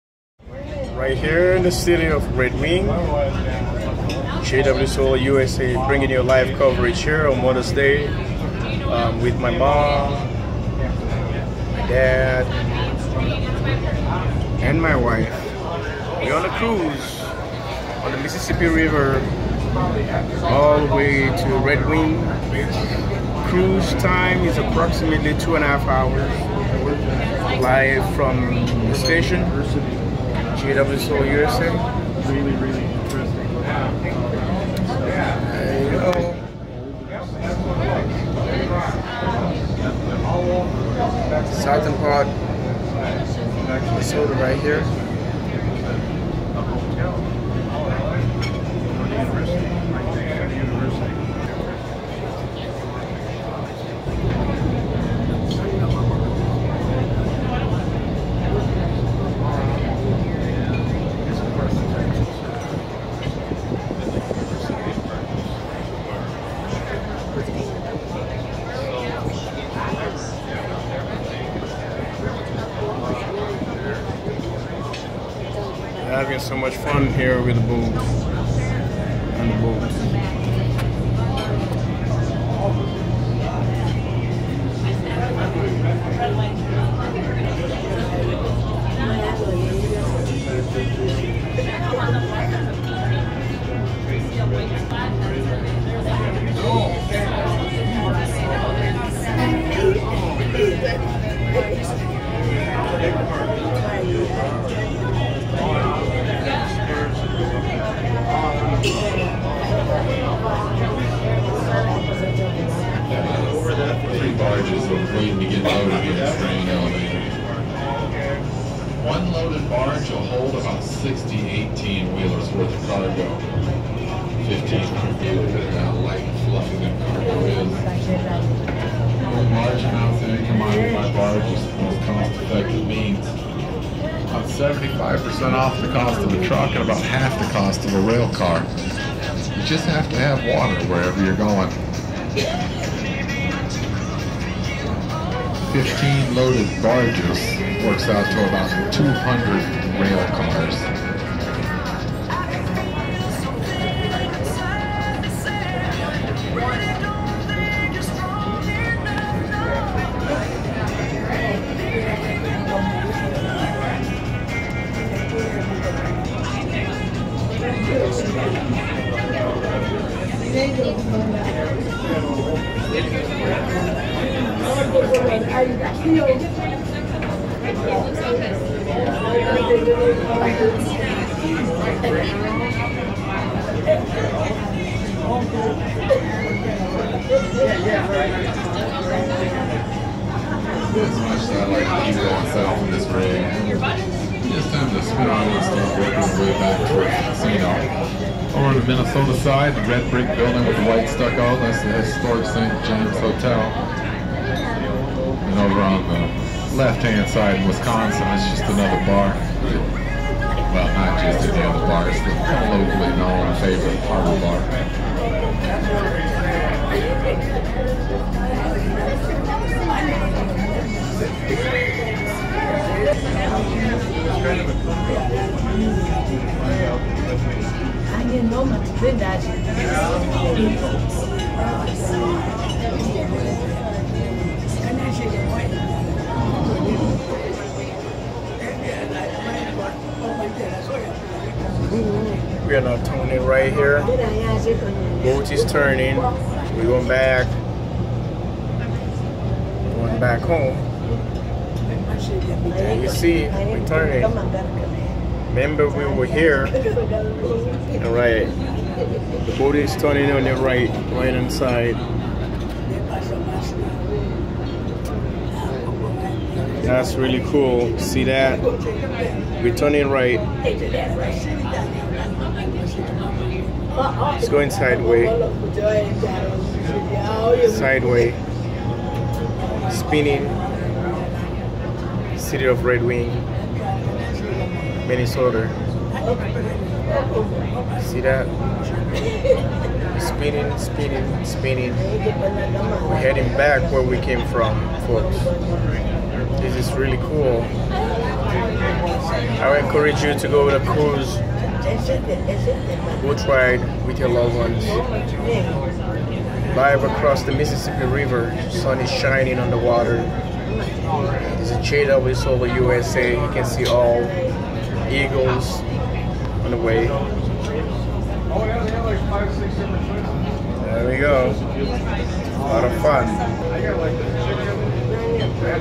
Mother's Day Cruise #3
I took my mother on a cruise on mother's day

PLEASE STAY TUNE FOR MORE VIDEOS AND INFORMATION ON THIS CHANNEL.

+jwsolarusa